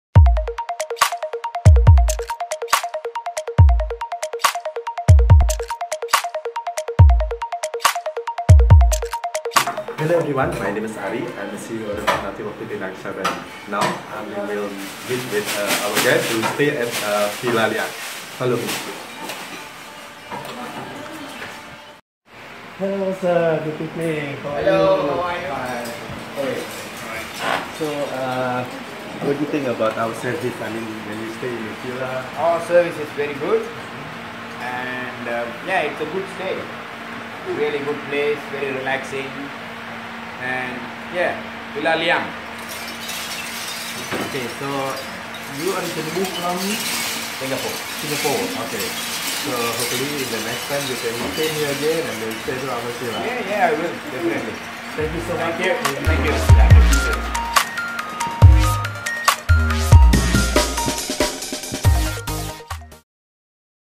Guest Coment at Villa Liang | Exclusively Managed by Nagisa Bali
Regarding the positive comments from our guest at Villa Liang about good service from our staff during handling guest.

CHECK OUR STORIES AT :
Villa activities: @nb.activities
Part of : @nagisa_bali_group

SYNC :
Lina Shimizu
Founder & CEO PT. Nagisa Bali